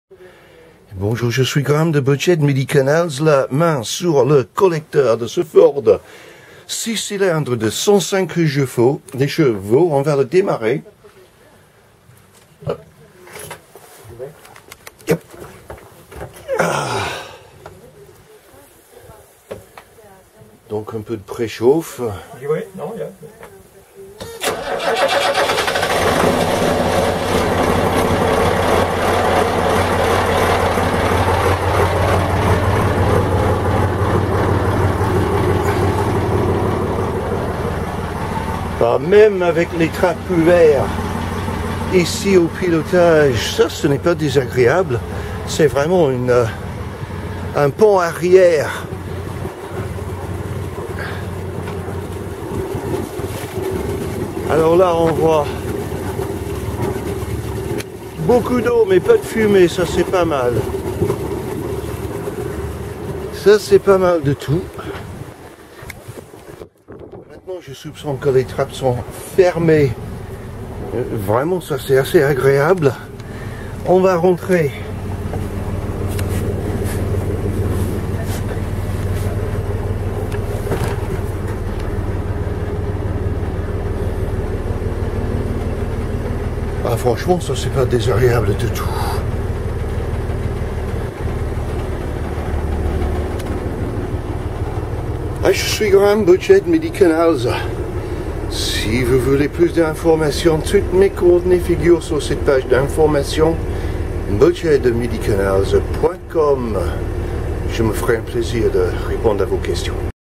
Dutch Motor Cruiser GLISSENVLET 1130 - Boatshed - Boat Ref#311616
Dutch Motor Cruiser GLISSENVLET 1130 for sale with Boatshed Midi Canals Photos and video taken by Boatshed Midi Canals